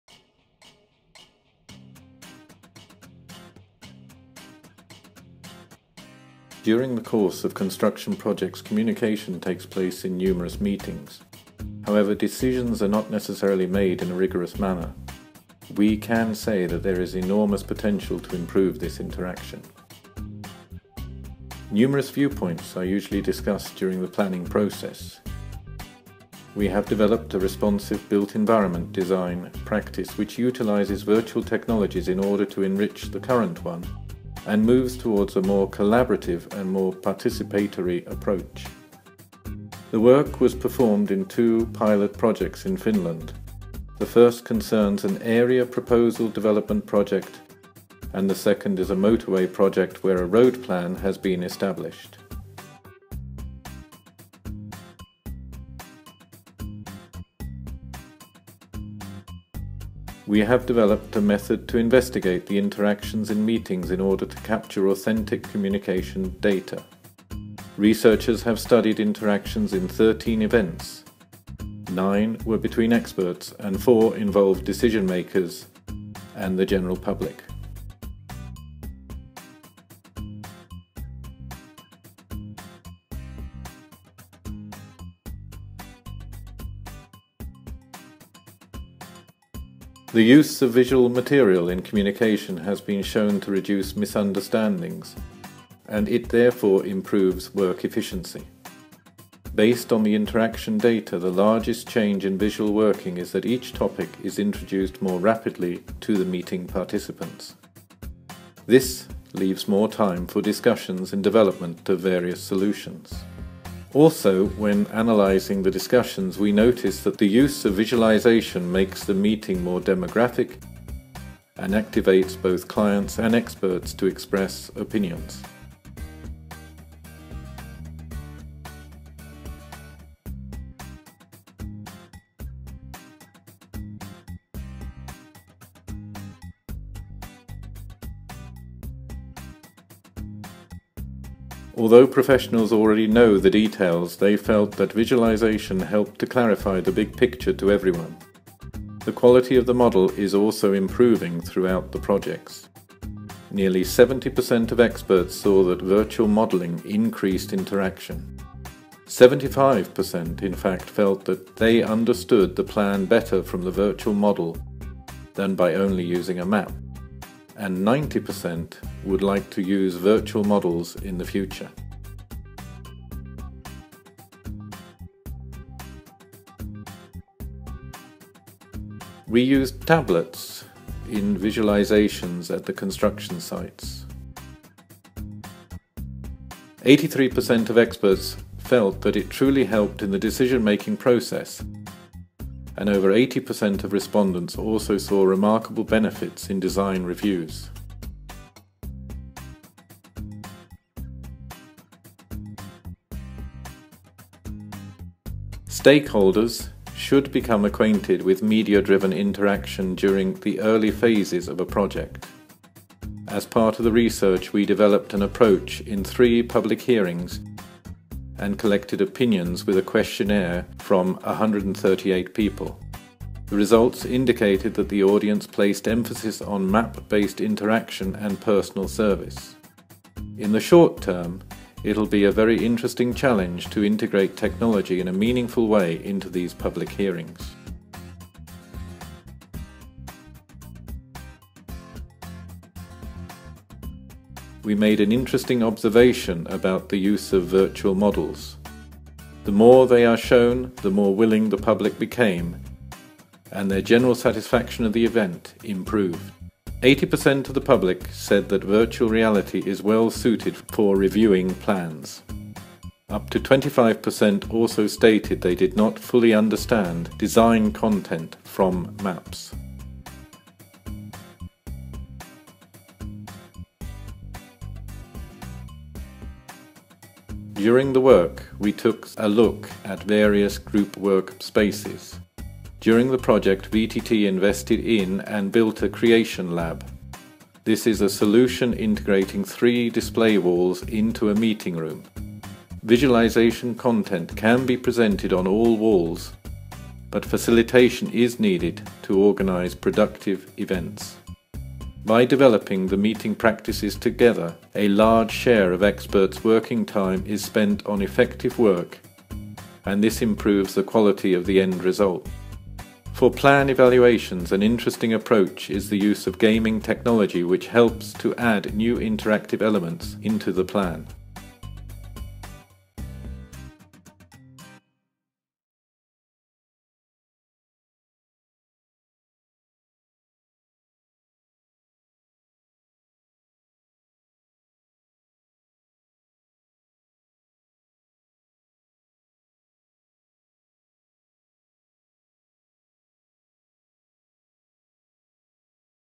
VIREsmart project results
VIREsmart project has investigated interaction and especially the importance of visualising data to various construction project stakeholders. This video introduces main research findings during the work.

- Project manager: Janne Porkka, VTT Technical Research Centre of Finland
- Participants: Tekes, City of Espoo, Finnish Transport Agency, Centre for Economic Development, Transport and the Environment, Sito Ltd., Vianova Systems Finland Ltd., Ramboll Systems Finland Ltd., Tampere University of Technology and VTT Technical Research Centre.
- Technology: Novapoint Virtual Map, Novapoint VDC Live and Unity3D.
- Narrator: John Kettle, VTT Technical Research Centre of Finland.
- Video: 2014, Janne Porkka, VTT Technical Research Centre of Finland